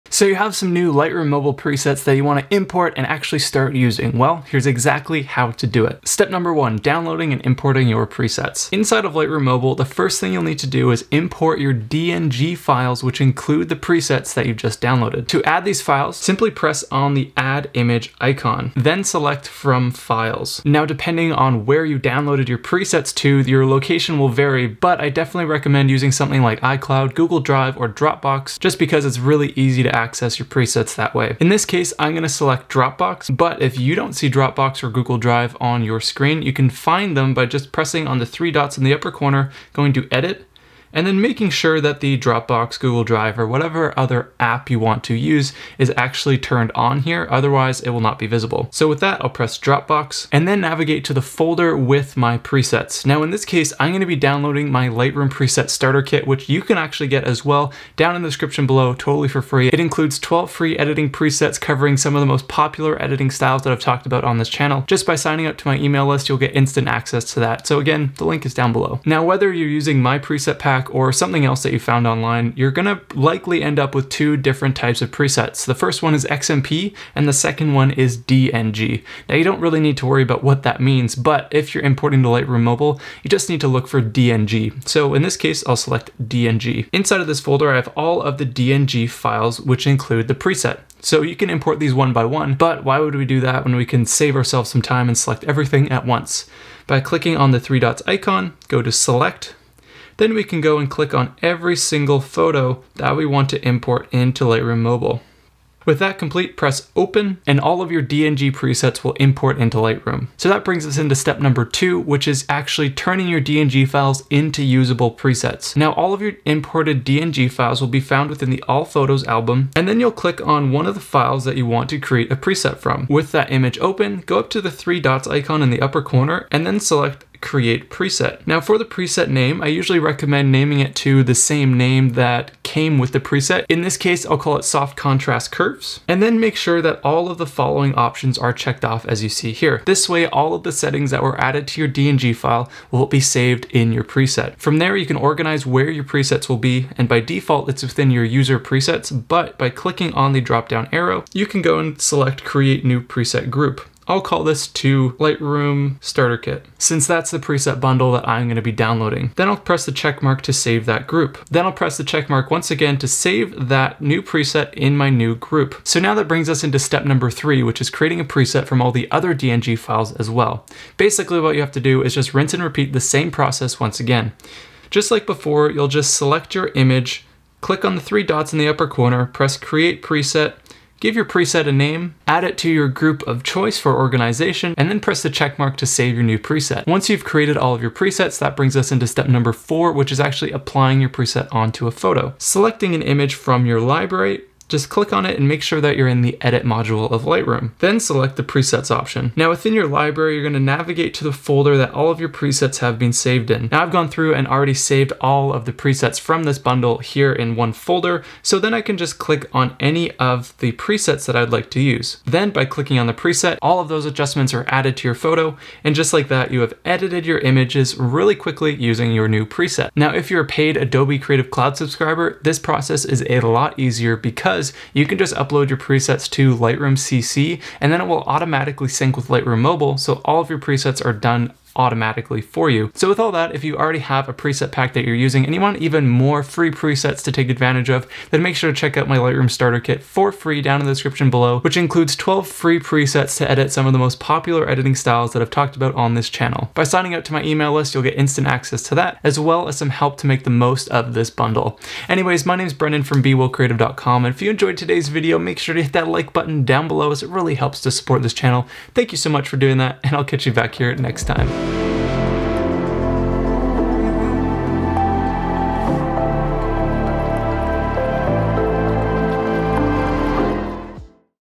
How To Import Lightroom Mobile Presets In 4 Easy Steps
If you have a new preset bundle for lightroom and lightroom mobile, importing your presets is the first thing you need to do. But if you are using the free version of Lightroom mobile, you can't just sync your presets between the desktop and mobile version of the program. Luckily, the four steps that I share in this video will allow anyone using the free version of lightroom mobile to quickly import their lightroom presets in just a few clicks. If you have a large number of presets, this can take a few minutes since you have to manually import each individual preset, however, with some of the batch selecting methods I outline here, you can somewhat speed up this process!

Timestamps
0:00 Importing Lightroom Presets
1:50 Saving DNG Presets In LR Mobile
2:48 Saving All Other DNG Files
3:10 Apply Your Presets To A Photo
3:42 Get 12 Free Lightroom Presets!